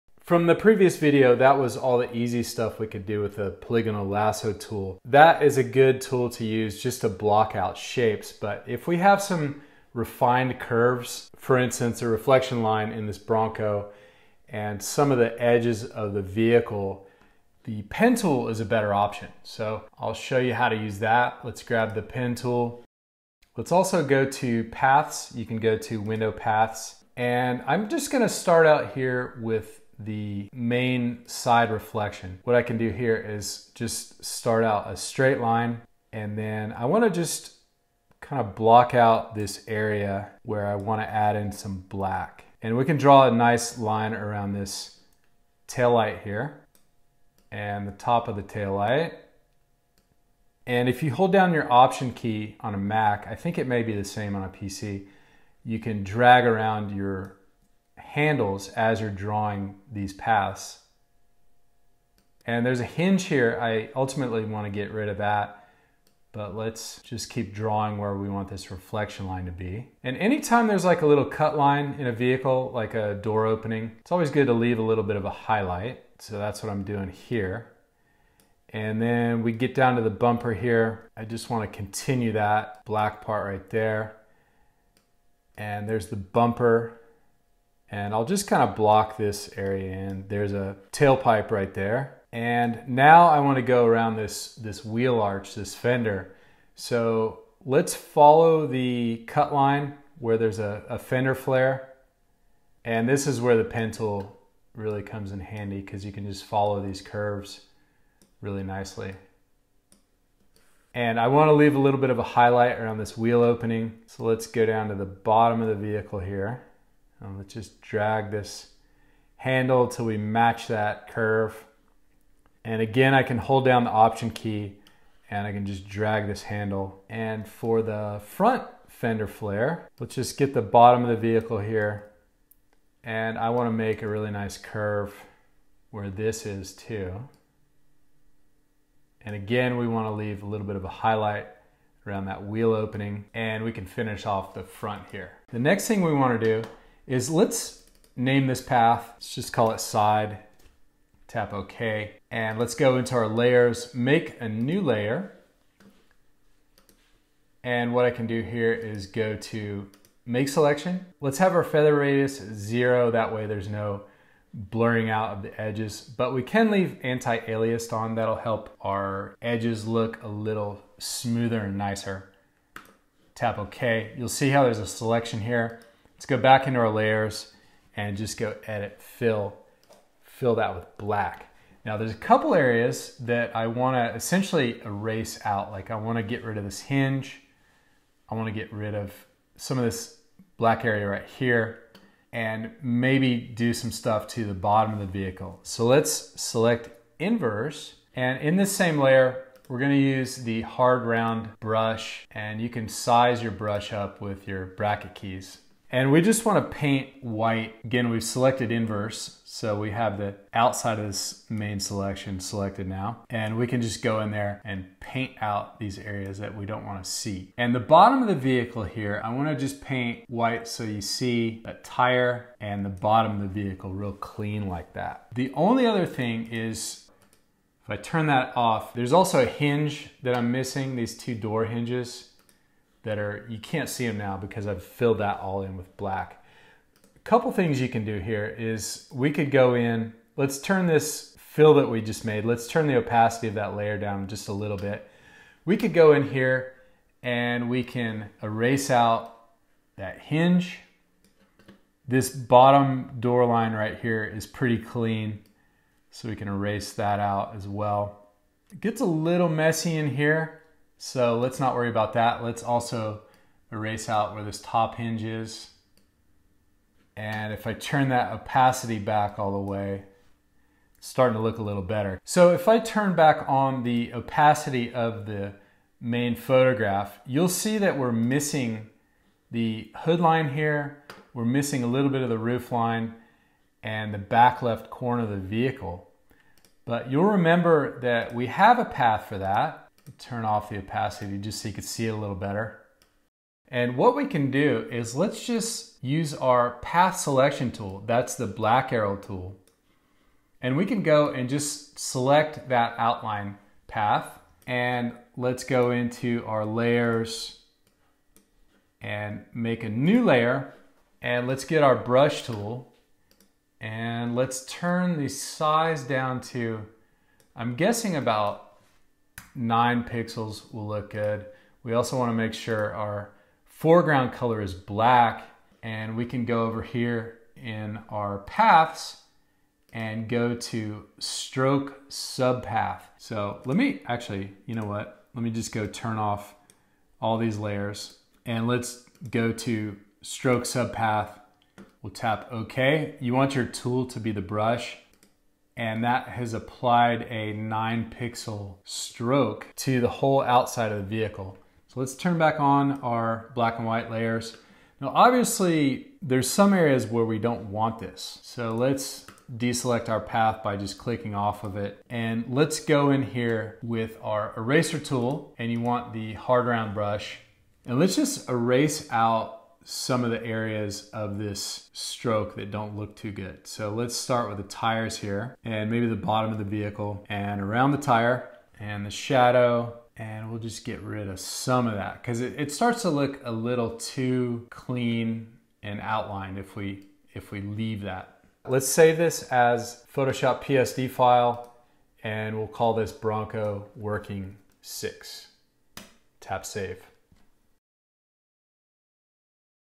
Pen Tool - Photoshop (Video 6) T-Shirt Design Workshop 4
Use the pen tool to improve your graphics in Photoshop. to see all the videos in this workshop.
T-Shirt Design Workshop 4 will show you how to make photo-based t-shirt graphics.